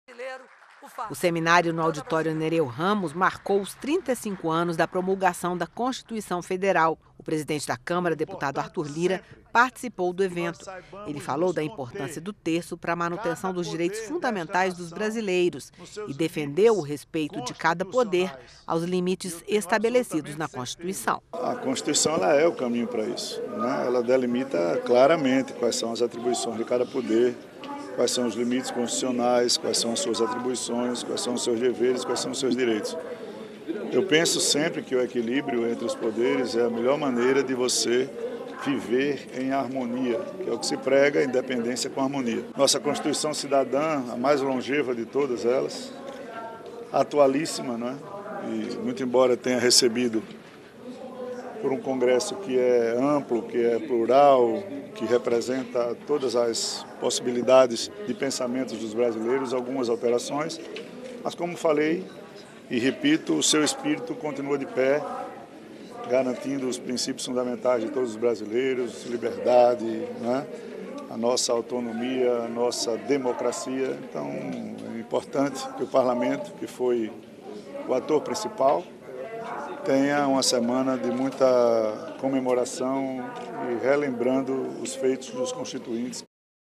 Lira defende limites dos Poderes estabelecidos na Constituição - 04/10/2023
A constituição brasileira está fazendo 35 anos. Em um seminário para comemorar a data, o presidente da Câmara, deputado Arthur Lira, defendeu o respeito à divisão de Poderes estabelecida na lei maior do país.